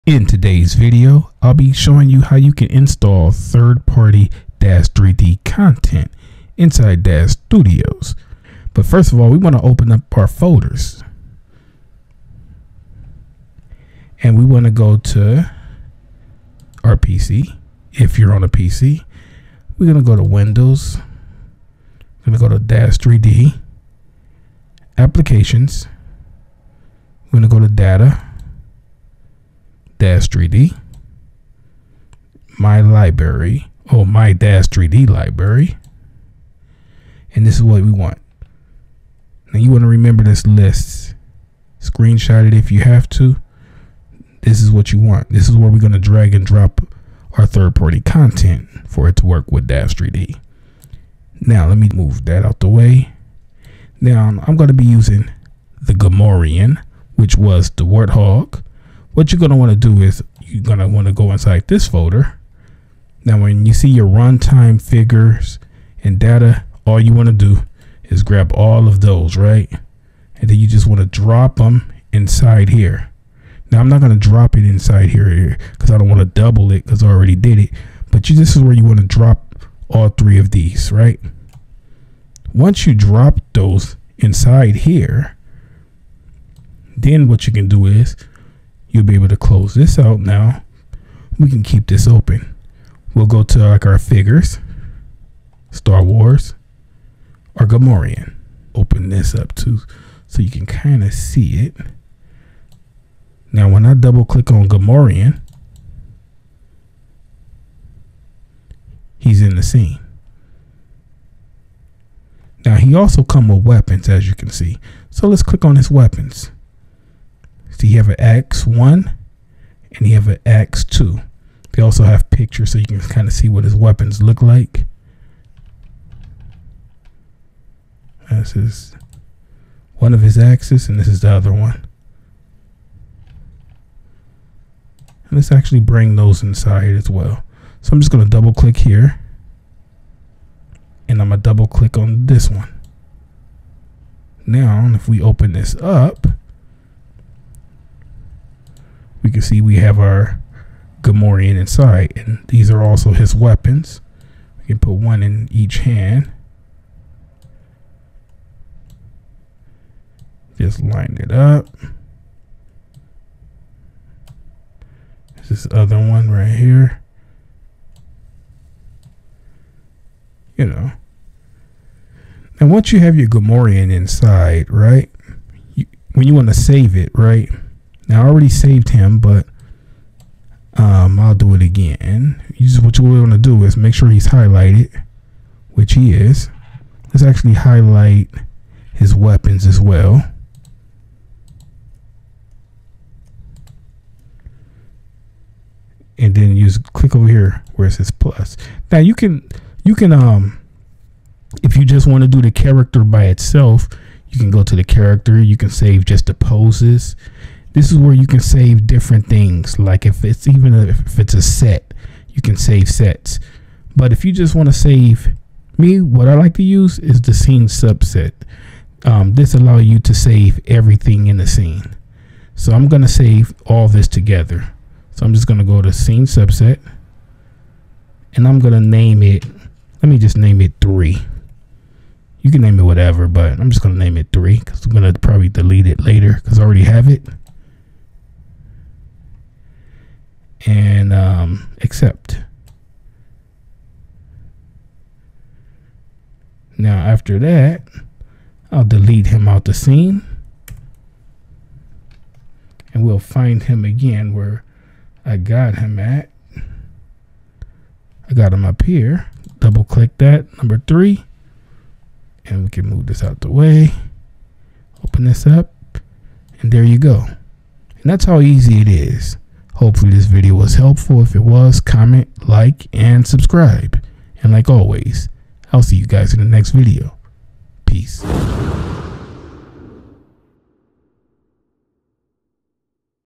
How To Install 3rd Party Daz3D Content (The EASY Way!)
In this user-friendly tutorial, I will guide you through the process of integrating third-party content into Daz3D.